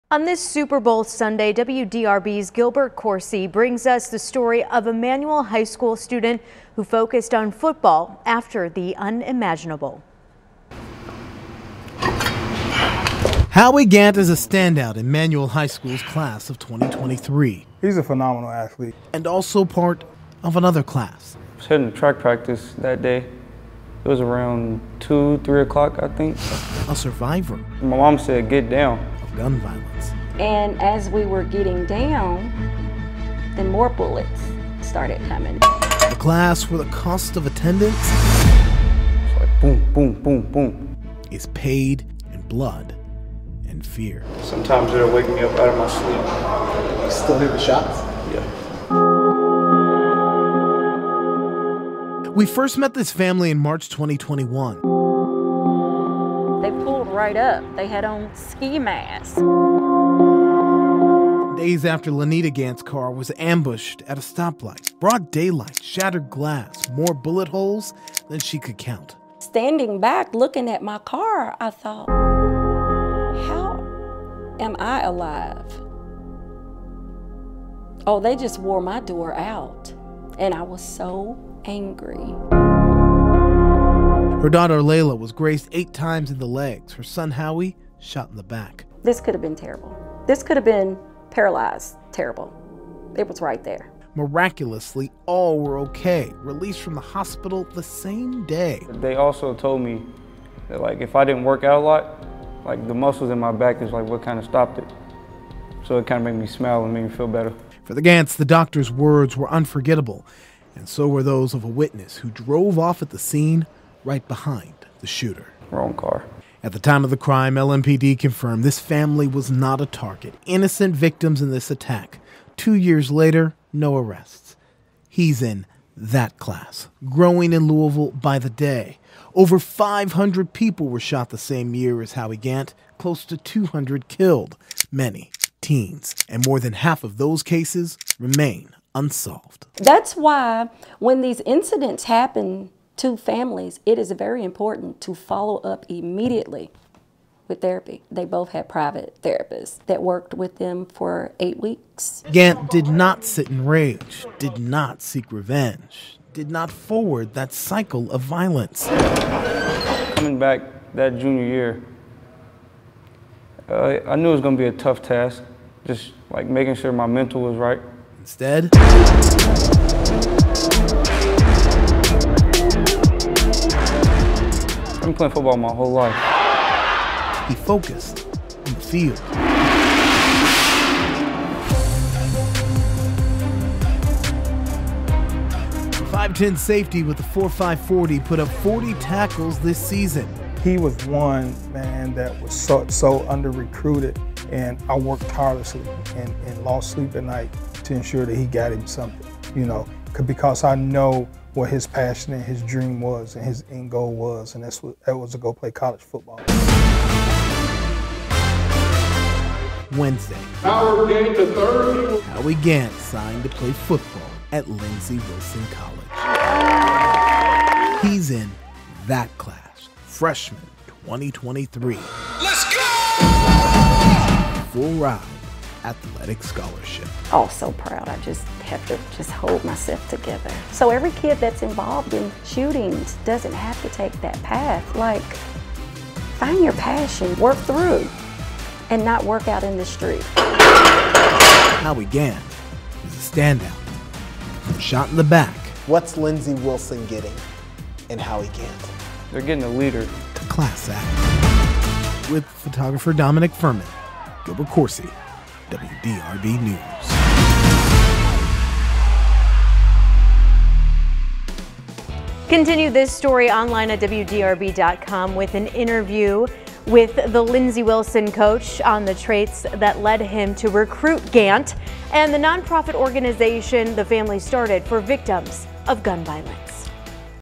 Manual H.S. football player shot in the back receives full athletic scholarship
A duPont Manual High School senior who was shot in the back at age 16 will graduate as the ultimate story of grace and grit.